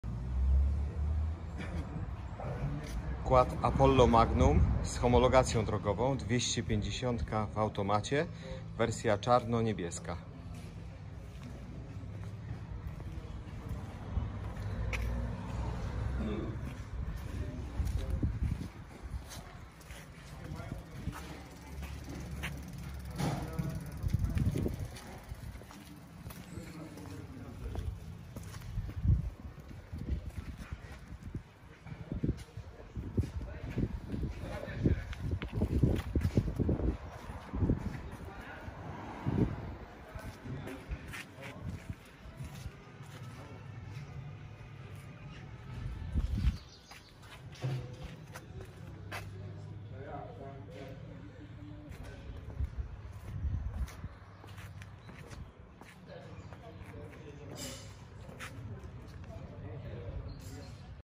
Salon2Kółka Quad Apollo Magnum 250 automat
Wersja quada Magnum z homologacją drogową. 250cm w automacie : wersja kolorystyczna czarno-niebieska